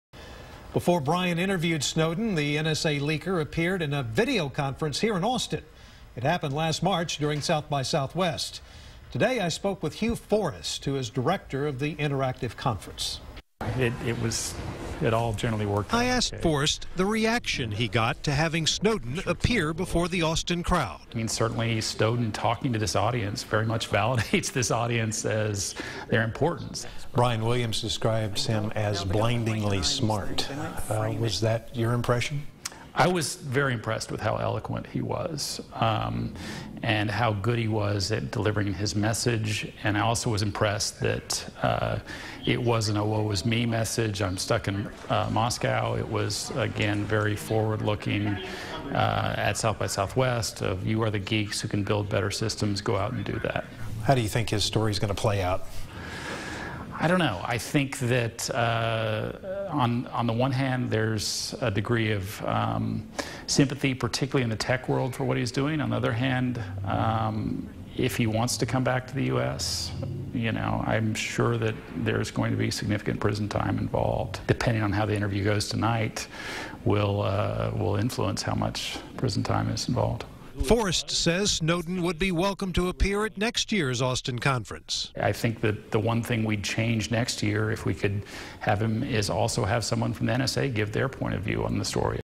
Before Brian Williams, Snowden spoke at SXSW
Two months before NBC's Brian Williams interviewed Edward Snowden, the NSA leaker conducted a virtual conversation with an overflow crowd at SXSW.

kxanaustin,kxanaustinnews